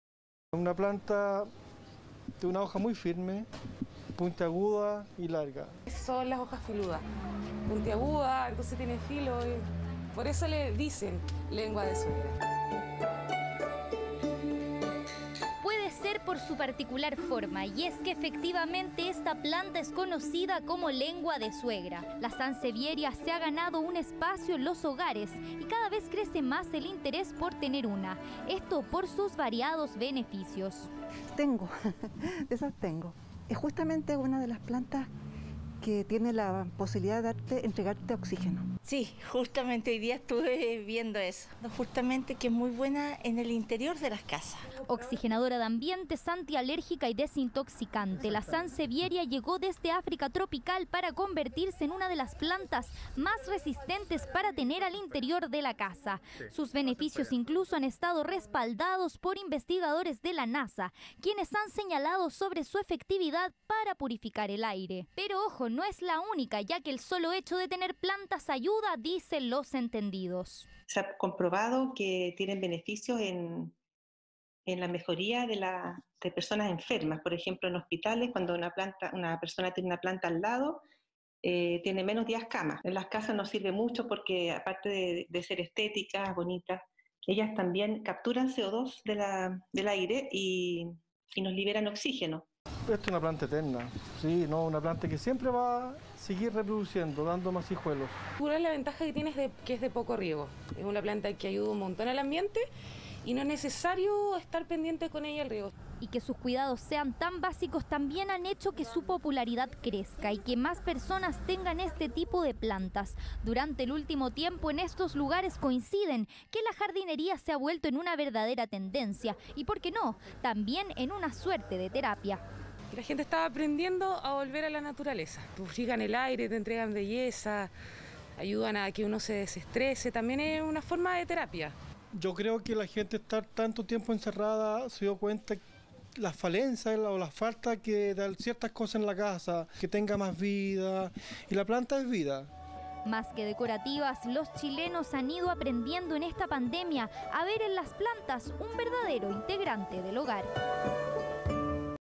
“Lengua de suegra”, la planta de interior que se populariza y gana adeptos en Chile
Más allá de su llamativo nombre, los fanáticos de la botánica han encontrado en esta especie un gran oxigenante para los hogares, así como su utilidad antialérgica, por lo que cada vez es más común verla en casas y departamentos de chilenos.